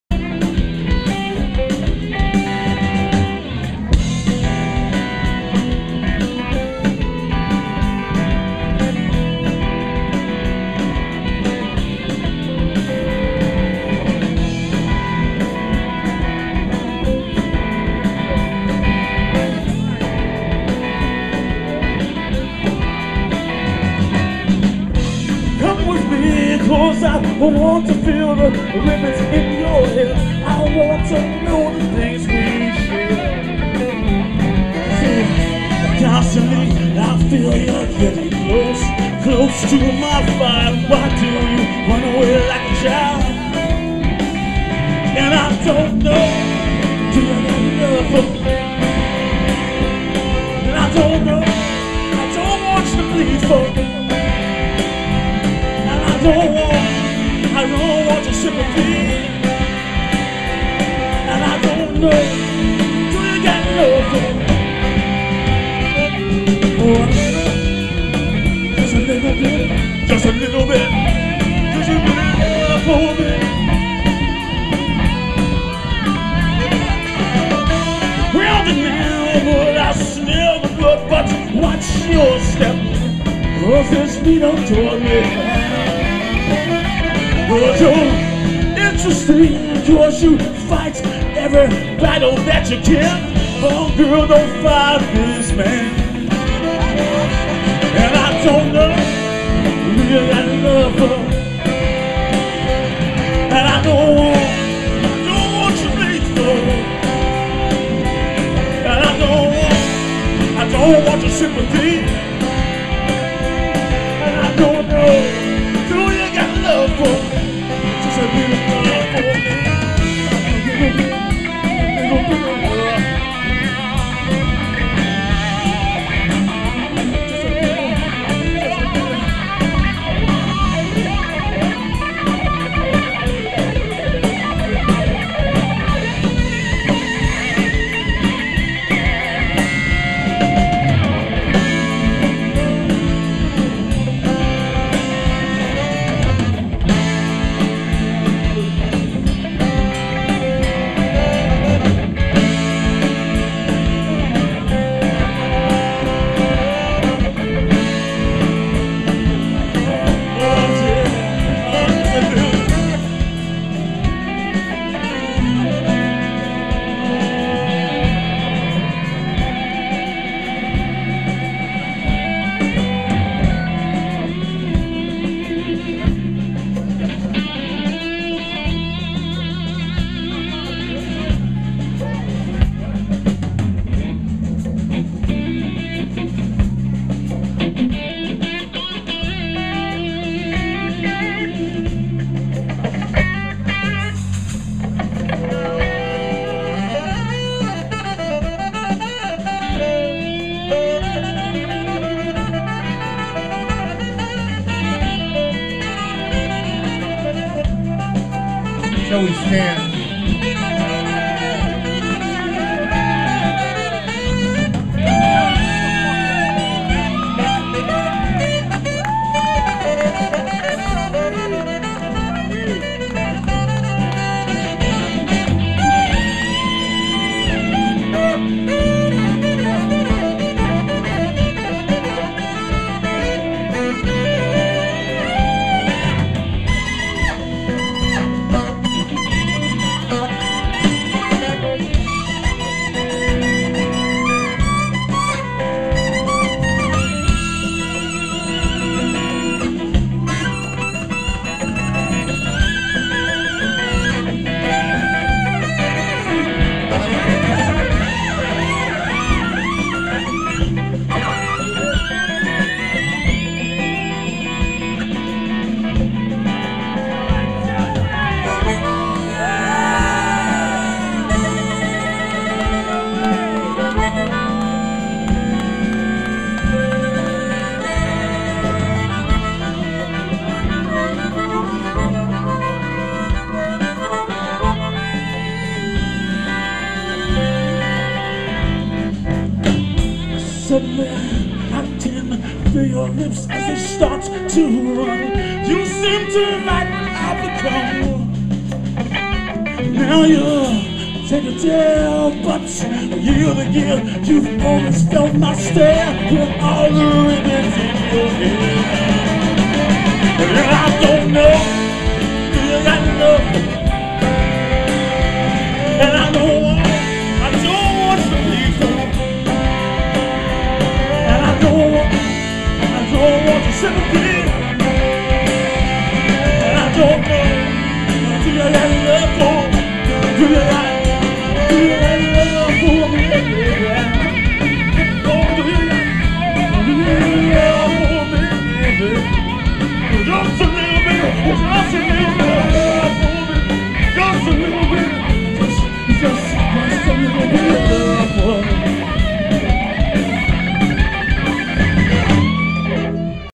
Nick Clemons Band, 11-18-11, Wonder Bar
Don't know the song title, but a pretty decent effort by Nick, who is the (sadly deceased) Clarence Clemons' son, and crew, with Joey Stann (ex-original Asbury Juke) blowing the horn in Asbury Park, NJ...a bit of a Bruce chase landed me here, but not a bad showing by Nick and friends...